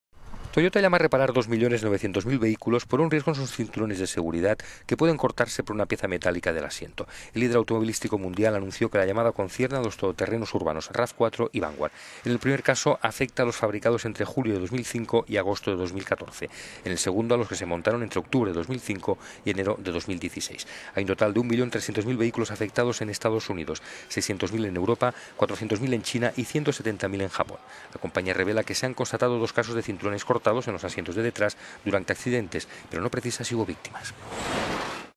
Toyota llama a revisión 2,9 millones de vehículos por un defecto en el cinturón de… - economy
Toyota llama a reparar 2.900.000 vehículos por un riesgo en sus cinturones de seguridad, que pueden cortarse por una pieza metálica del asiento. El líder automovilístico mundial anunció que la llamada concierne a los todoterrenos urbanos RAV4 y Vanguard. En el caso de los RAV4, afecta a los producidos entre julio de 2005 y agosto de 2014 y fueron vendido en todo el mundo. En el de los Vanguard, a los que se montaron entre octubre de 2005 y enero de 2016.